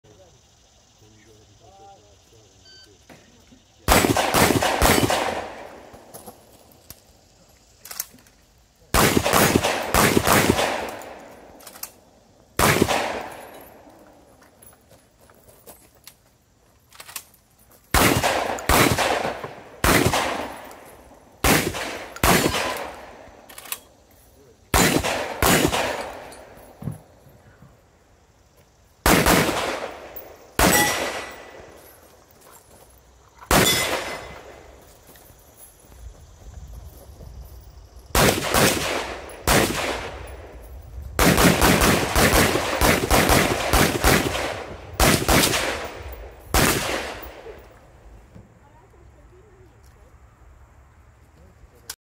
Zoo City Armory 3-Gun, 95 Degree Day and the RIA is Still Acting Up
Big thanks to Callaway Ballistics, Primary Arms, & Triggertech, everything was going great, the shotgun just did not want to go bang.  Here is the first stage of the day.